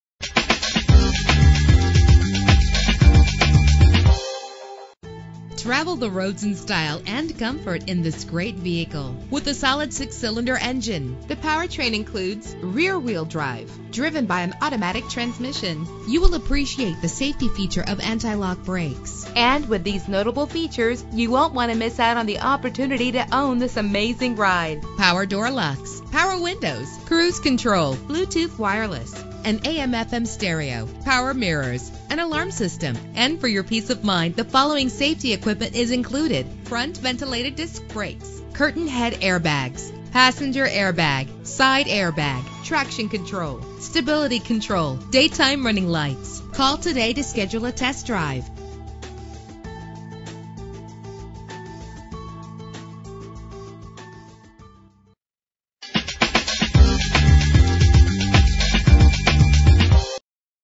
2015 Dodge Challenger - Ocala FL
Year: 2015
Make: Dodge
Model: Challenger
Trim: SXT / R/T
Engine: 3.6 liter 6 cylinder 24 valve
Transmission: 8-Speed Automatic
Color: Bright White Clearcoat
Mileage: 0
Address:
3440 South Pine Avenue